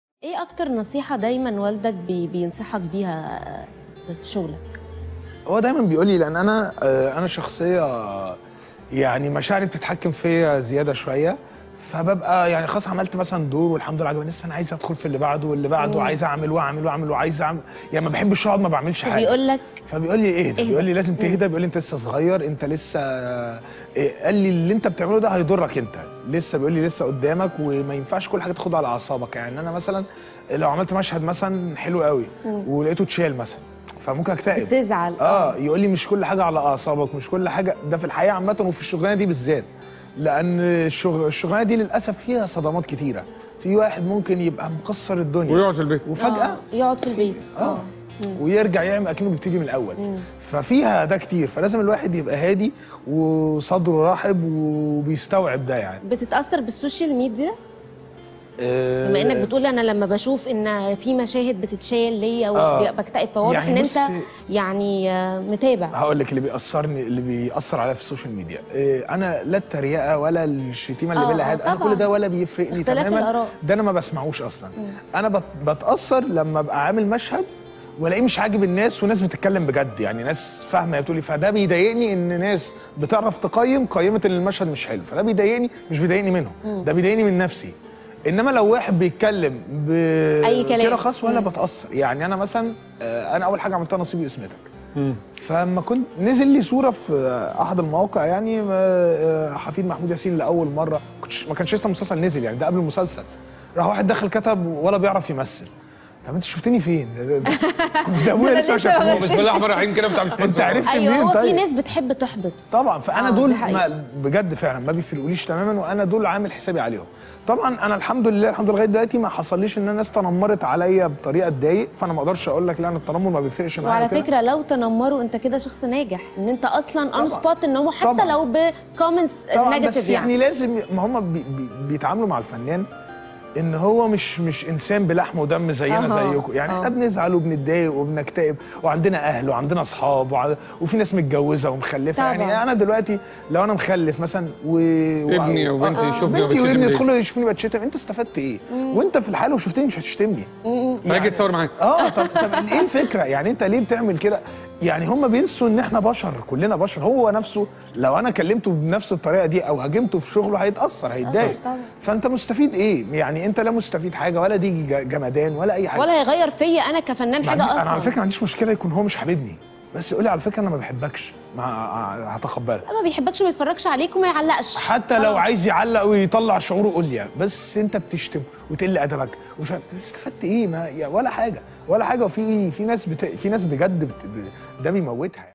"اللي بتعمله هيضرك".. محمود ياسين يكشف عن نصائح والده له🤔
وشوشة موقع الفن الأول في مصر يحتوي على العديد من الأخبار الخاصة بنجوم الفن